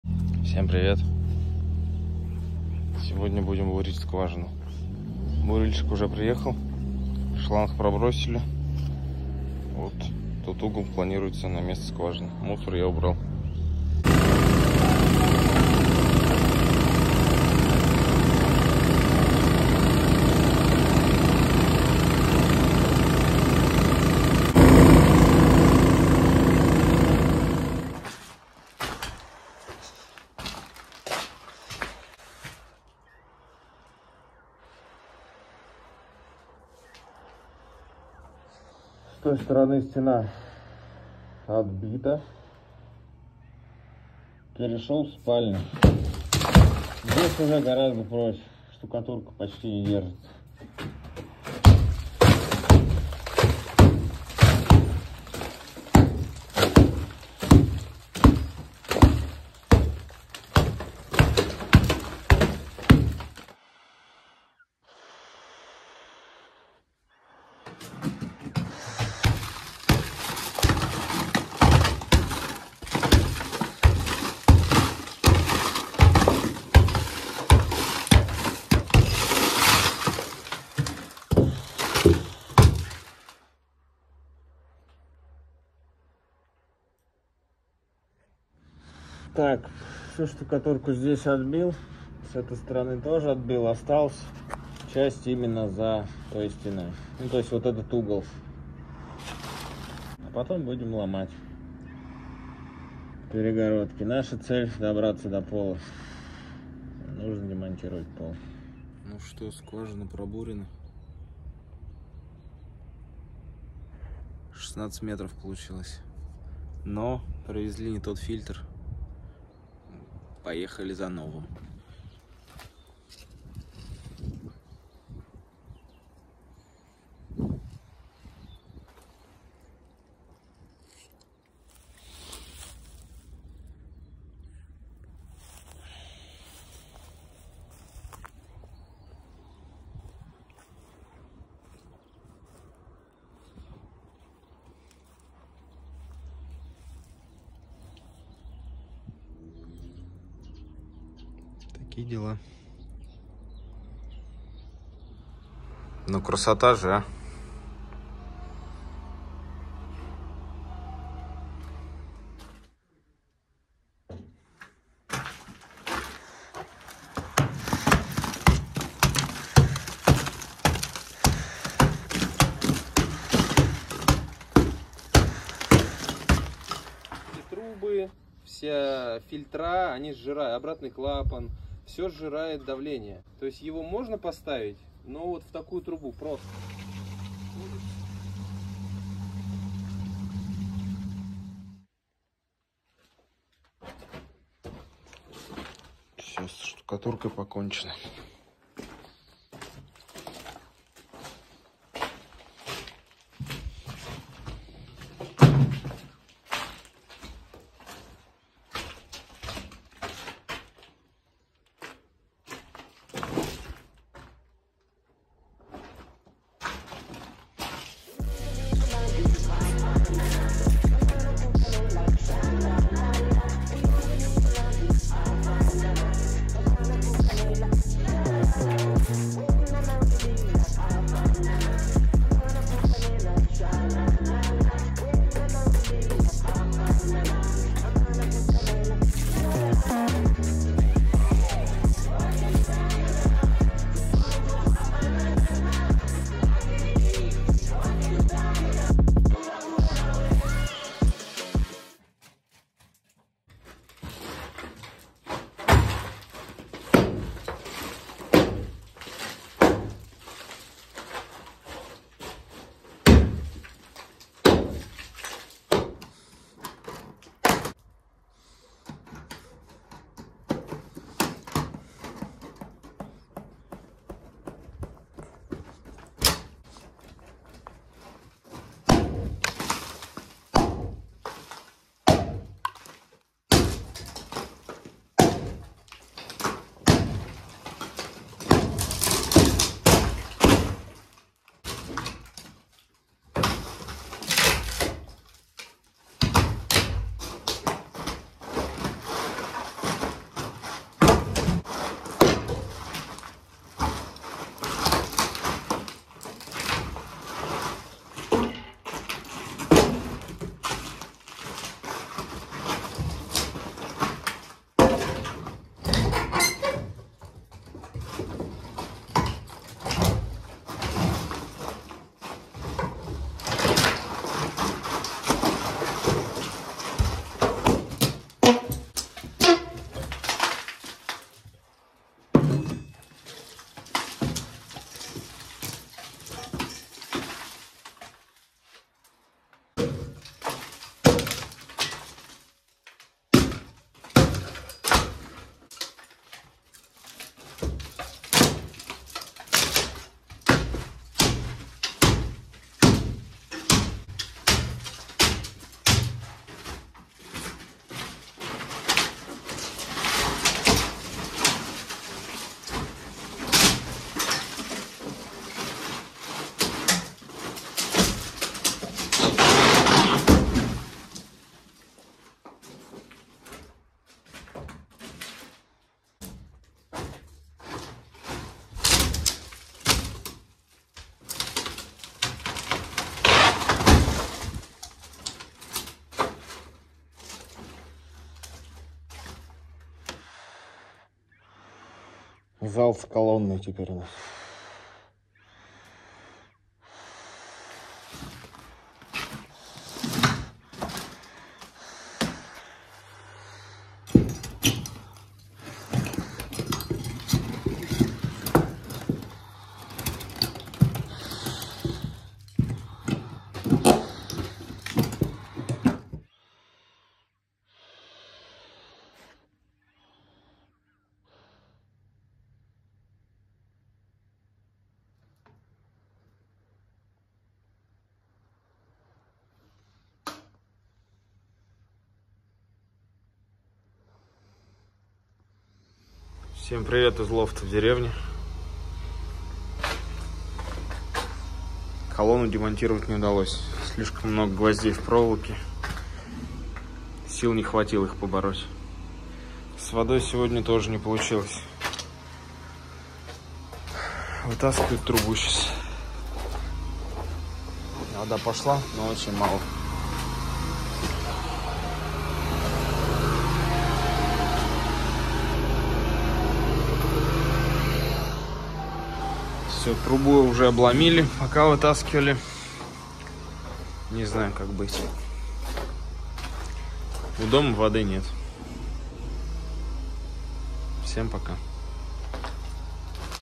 СКВАЖИНА БЕЗ ВОДЫ И ДОМ БЕЗ СТЕН
Пробурили скважину - да не тут-то было…

Ломаю перегородки в доме

Всем мир ✌️